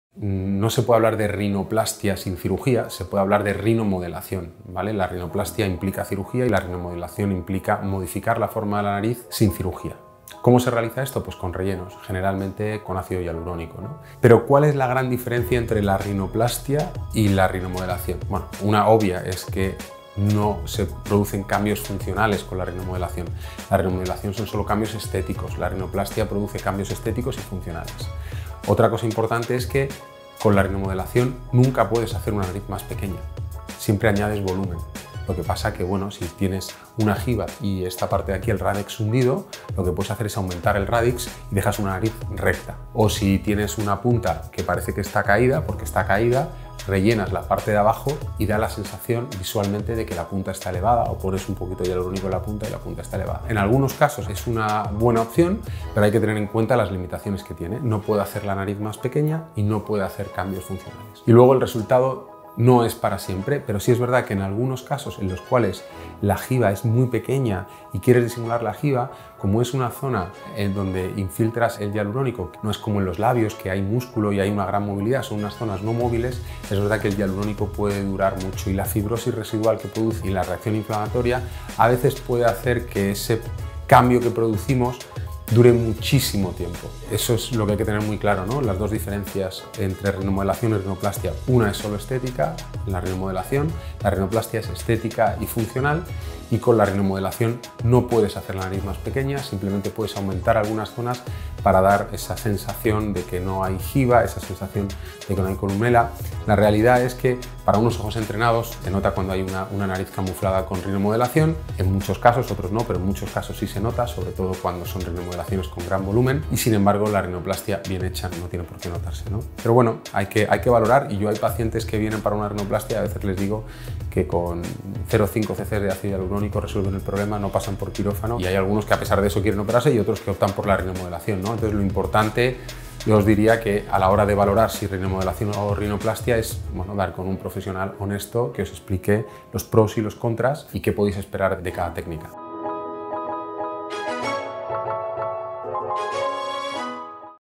Rinoplastia y rinomodelación sus diferencias
En este vídeo aclaramos la dudas sobre las diferencias entre rinoplastia y rinomodelación. Los procedimientos tienen la misma función, mejorar el aspecto de la nariz, pero para ello se usan técnicas diferentes con resultados finales y duraderos muy diferentes.

Visita nuestra página web para conocer más acerca de la cirugía de rinoplastia en Mallorca y Barcelona: